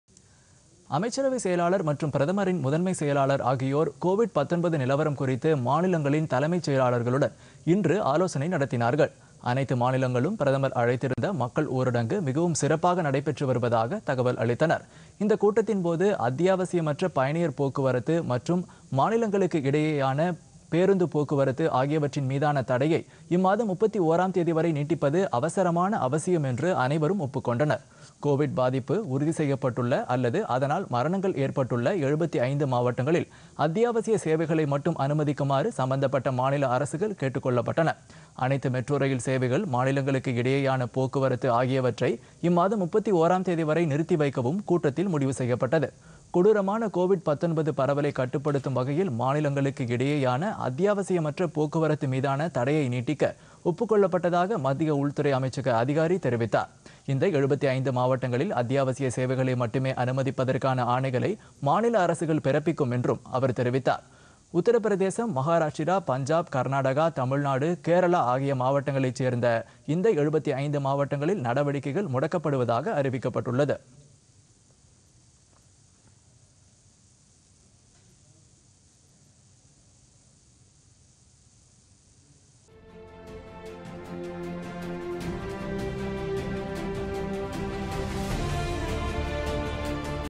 நாட்டின் 75 மாவட்டங்களின் நடவடிக்கைகளை முடக்க முடிவு
கோவிட்-19, கட்டுப்பாடுகள் நடவடிக்கைகள் குறித்து மாநில தலைமைச் செயலாளர்களுடன் அமைச்சரவை செயலாளர் மற்றும் பிரதமரின் முதன்மை செயலாளர் ஆலோசனை